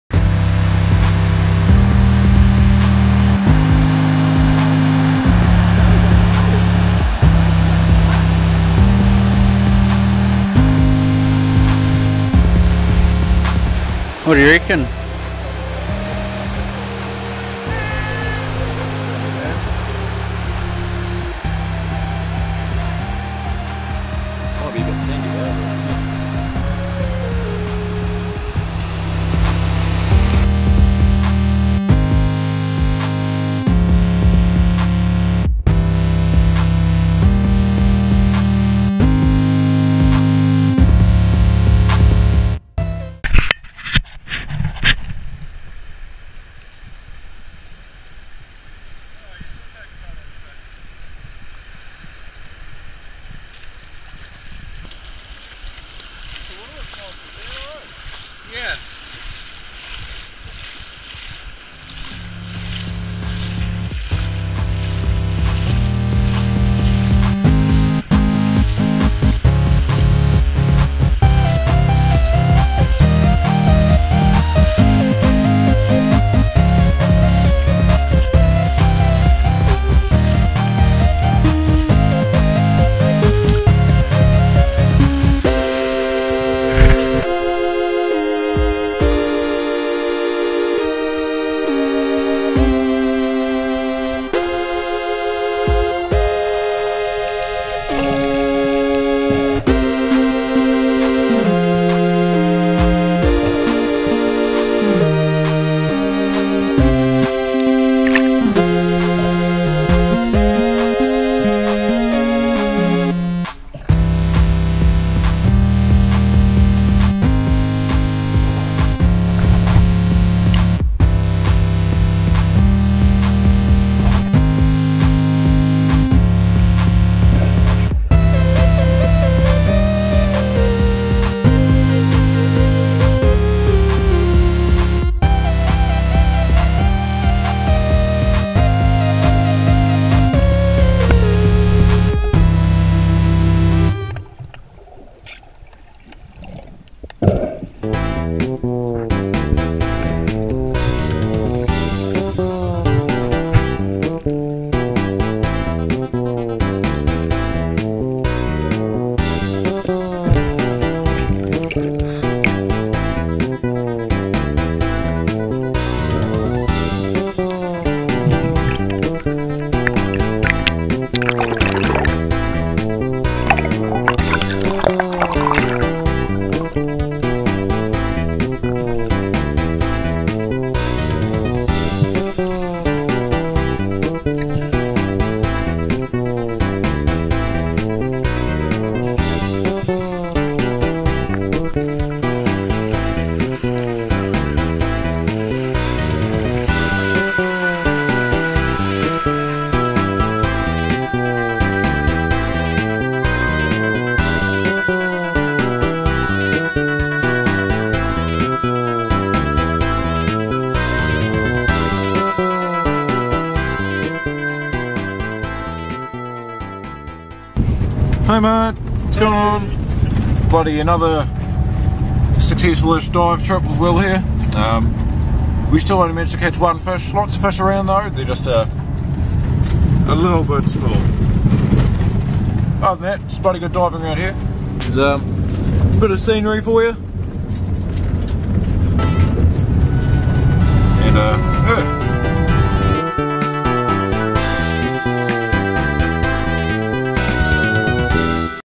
Spearfishing Murdering Beach
Went for a wee splash with Mike at Murdering Beach. Didn't live up to name of the beach with only one fish shot. Beautiful day out.